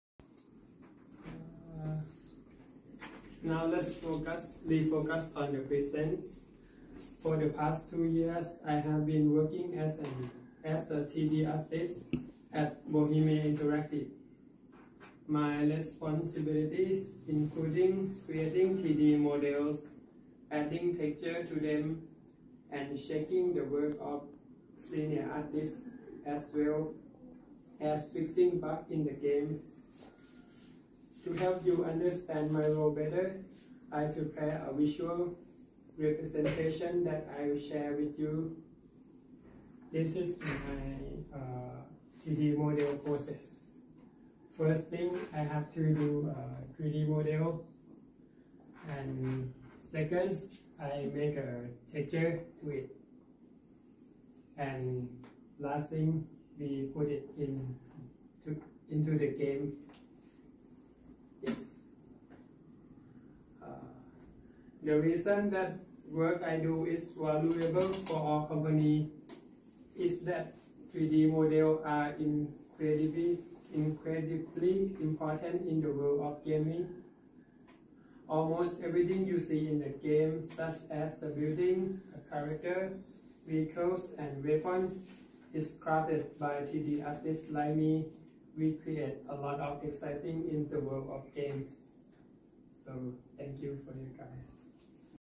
15 October 2023
A Business English student in his final presentation class at Bohemia Interactive. Teacher Missy.